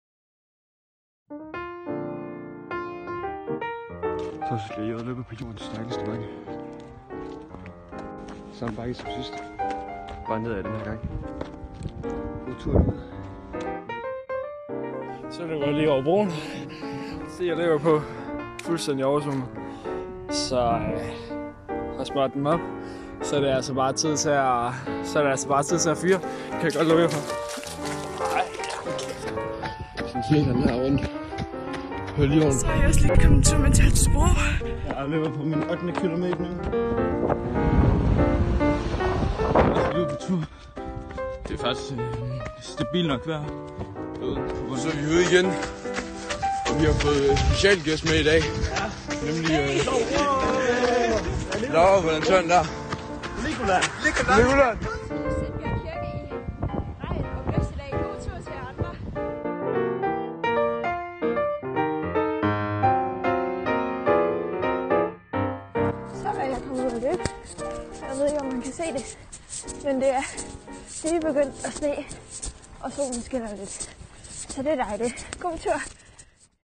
Marathonholdet
Der bliver tilbagelagt mange km fra marathonholdet rundt omkring i landet, da de stadig træner op til at løbe et marathon. Det er så sejt – og skønt at de opmuntrer hinanden med små videoklip.